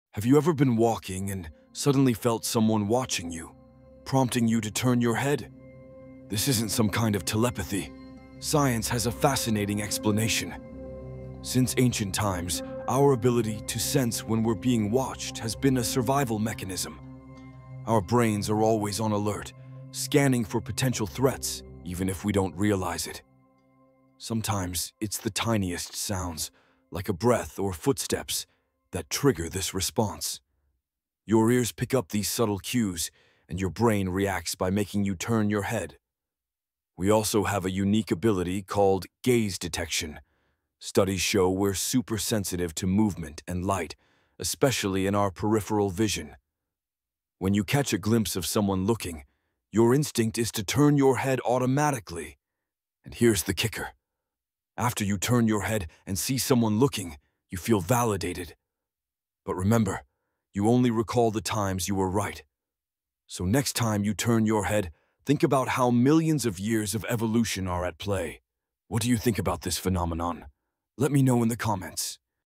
Why You AUTOMATICALLY TURN YOUR HEAD When Someone Looks? Science EXPLAINS The Weird Phenomenon! 🤯
Do you want to know why you automatically turn your head when someone looks? This is a fascinating psychological and biological phenomenon, and this Short video will use science to explain the phenomenon! Don't think of it as a superpower, but understand it as a powerful survival mechanism of the brain.

Learn about the "ancient alert system" and the role of sophisticated hearing in making you automatically turn your head. The video also touches on confirmation bias – the reason you believe the automatically turn your head response is always accurate. Science explains the phenomenon to help you better understand how your mind and body react to your surroundings. Watch until the end to discover the truth behind the gaze!

📢 CTA: How often do you automatically turn your head?